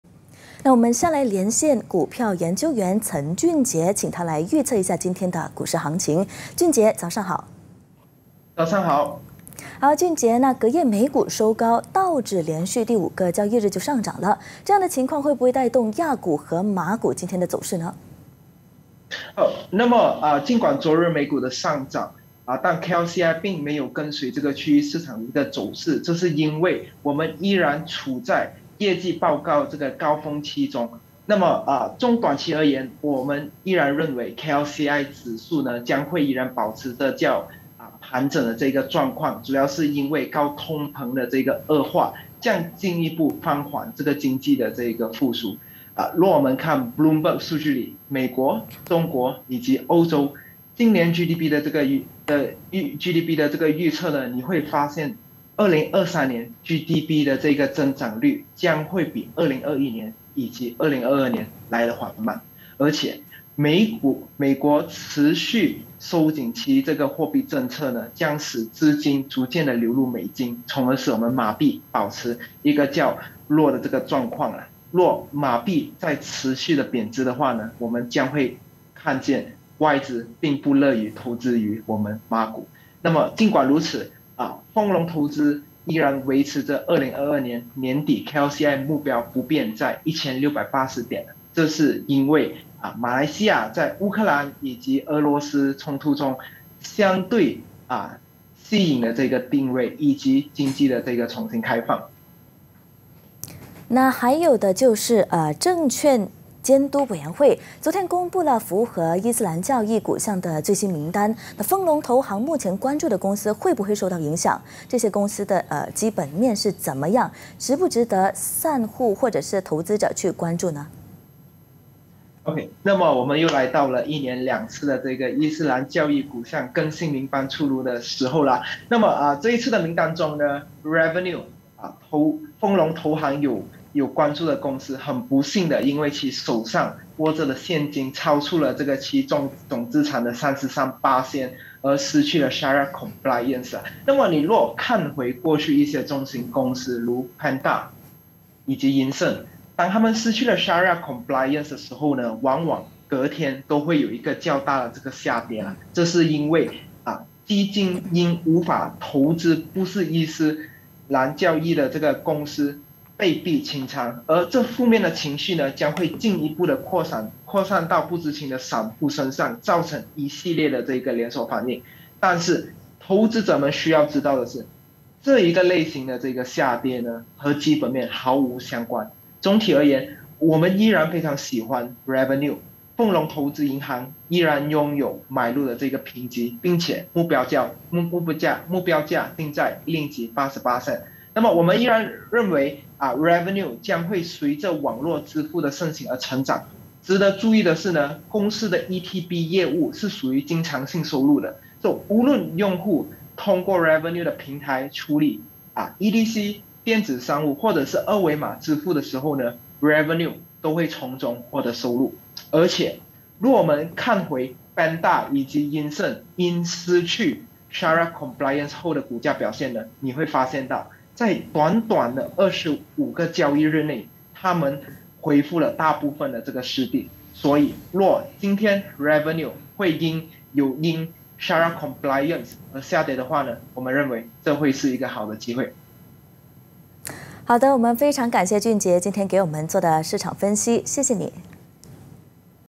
《股汇动态》伊教义股项新名单出炉 散户可"捡漏"?【2022.05.27 八度早新闻】
八度早新闻《股汇动态》
【伊教义股项新名单出炉  散户可"捡漏"?】
隔夜美股收高是否会带动马股今日走势？伊斯兰教义股新名单出炉，散户或投资者有"捡漏"机会吗？
嘉宾：岑俊杰 丰隆投资银行零售分析员

【今日主播】杨莉莎